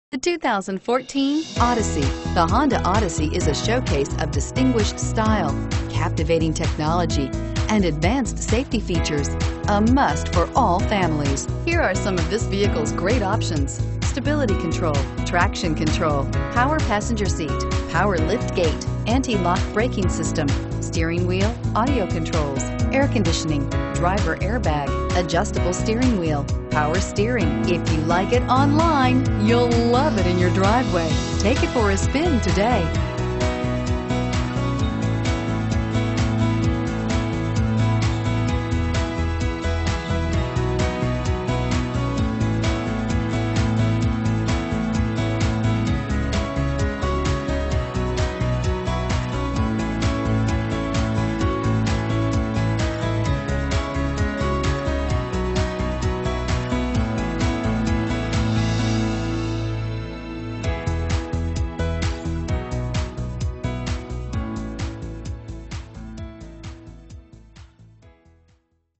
2014 Honda Odyssey Buford GA 470243
This 2014 Honda Odyssey 5dr 5dr EX-L Van . It is equipped with a Automatic transmission. The vehicle is POLISHED METAL with a GRY LEATHER Leather interior. It is offered with a full factory warranty. -